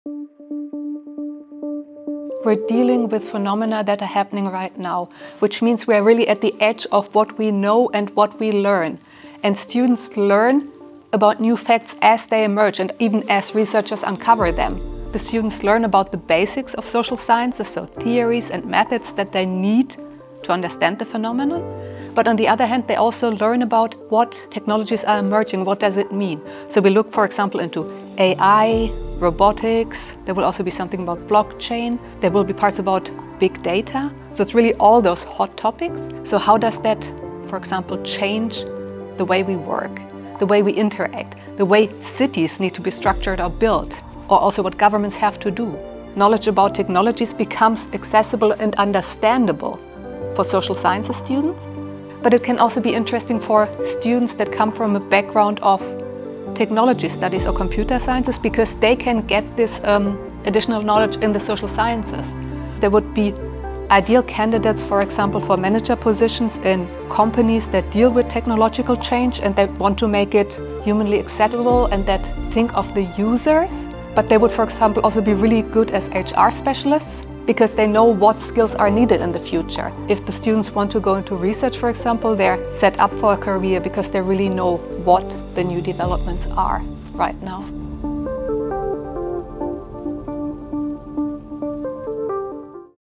👁️ Digital Social Science – LUT University
In the Master's Programme in Digital Social Science at LUT University, you will learn to understand traditional and emerging digital technologies and their societal impacts.

After graduation, you will know how to combine digital data with the analysis of societal and policy processes and human practices, offering valuable insights to business enterprises, public or third sector organisations.